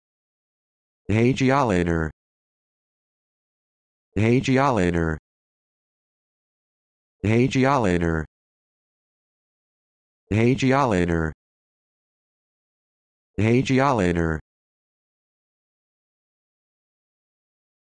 How to Pronounce Hagiolater
This video shows you how to pronounce Hagiolater